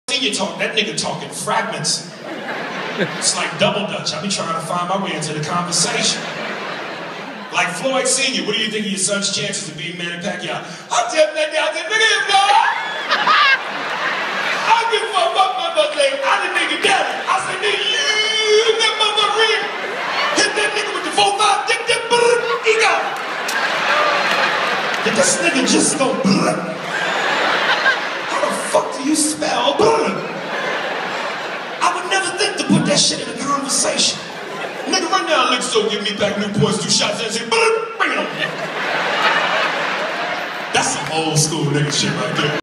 Aries spears ripping on roger mayweather
Spears cracking on roger mayweather.   Detroit opera house. Dec 8,2012